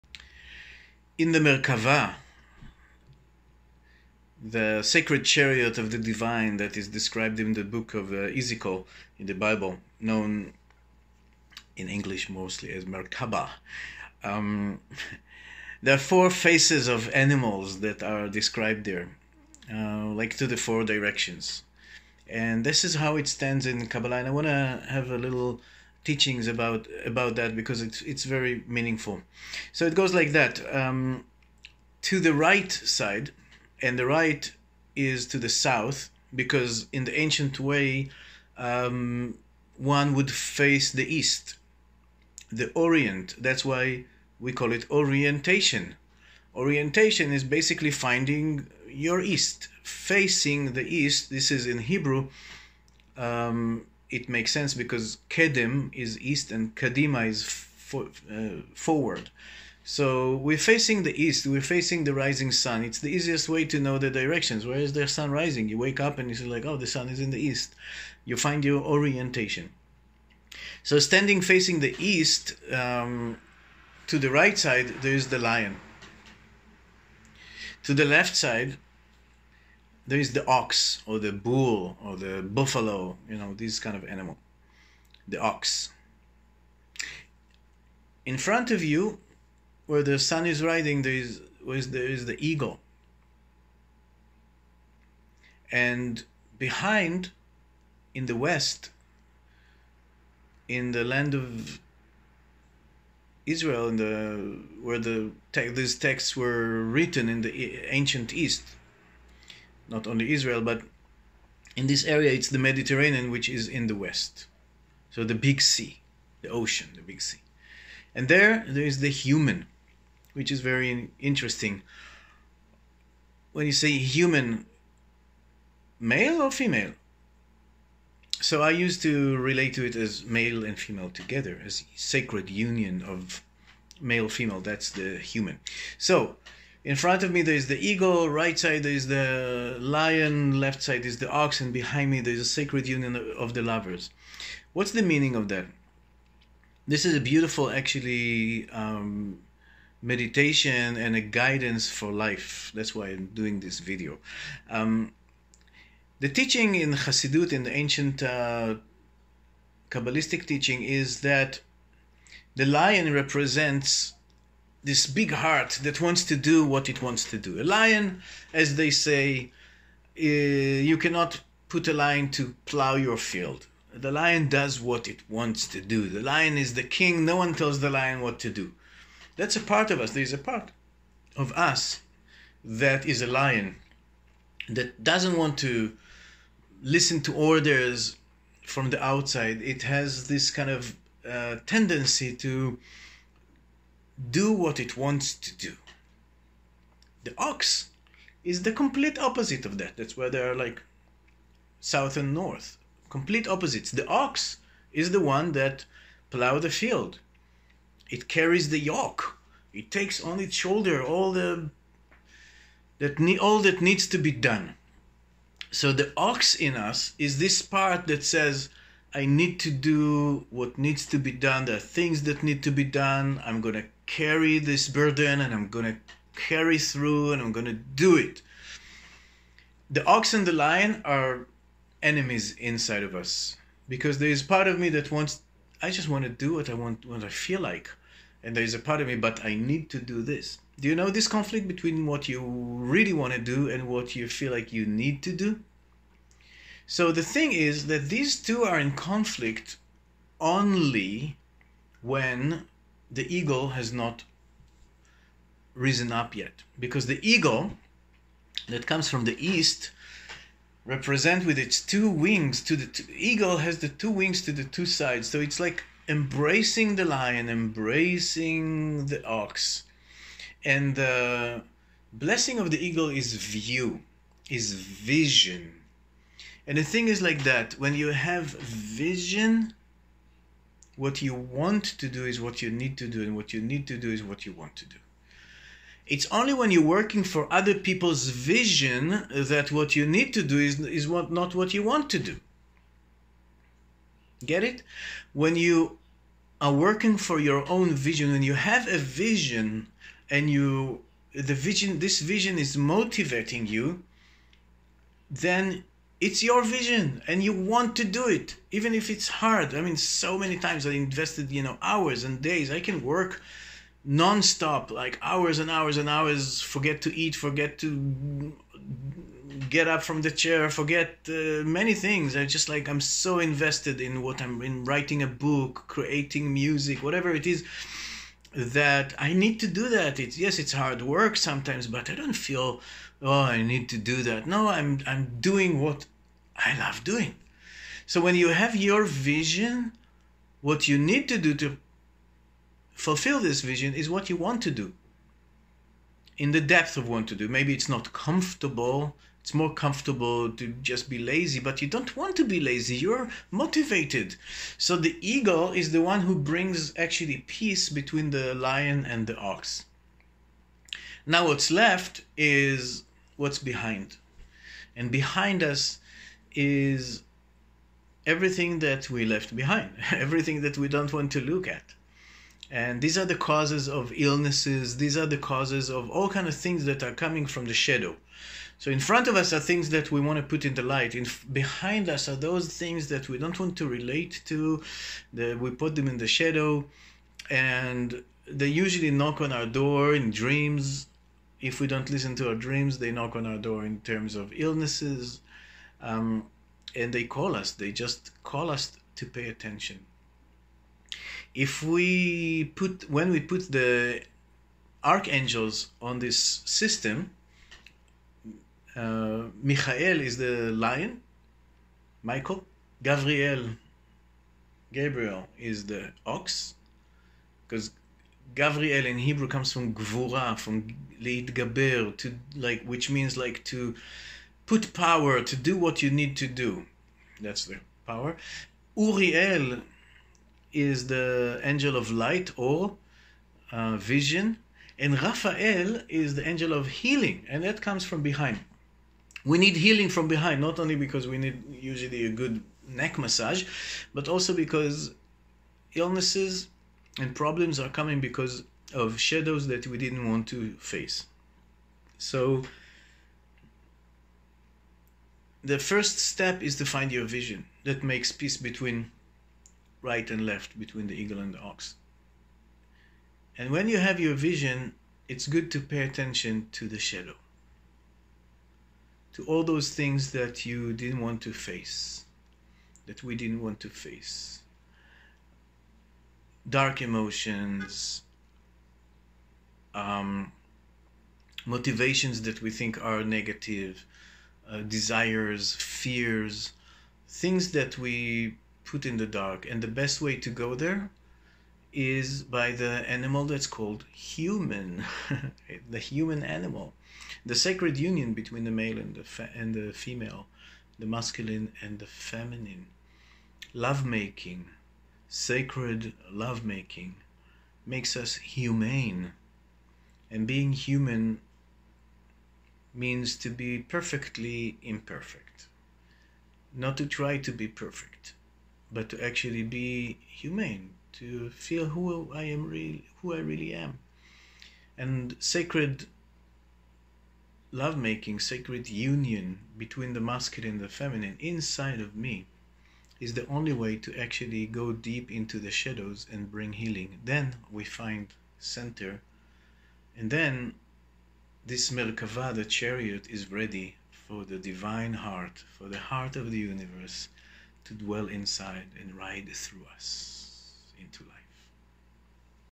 The 4 Sacred Animals of the Merkava in our Inner Work
Pele giving a short and powerful Kabbalistic teaching on the inner work of the Merkava and how to heal the conflict between what you Want to be doing and what you feel like you Have to be doing